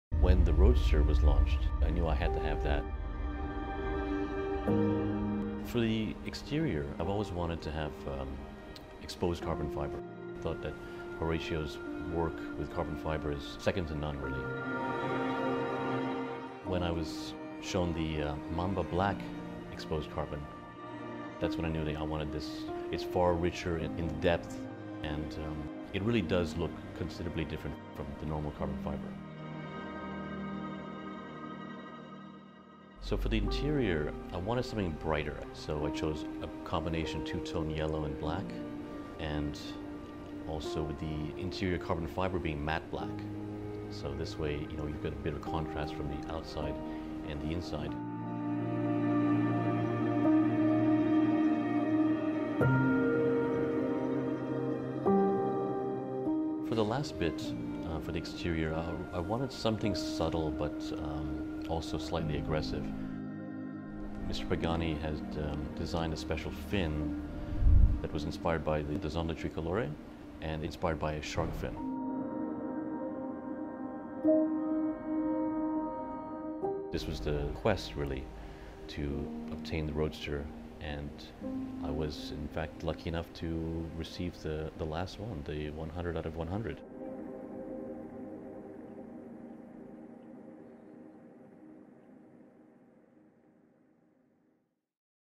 Huayra Roadster- 100 out of 100
Welcome to Big Bucks!

✓ 100 out of 100 Pagani Huayra Roadsters, mission accomplished!

A little more than 3 years from the worldwide debut, all the 100 Huayra Roadsters have been successfully delivered to their lucky owners, thanks to Pagani Team's strong commitment and passion to work.

What a wonderful journey it was! 100 specimens highly different from each other and highly customized according to the personal taste of the owner, and this one is exceptional. Have a look!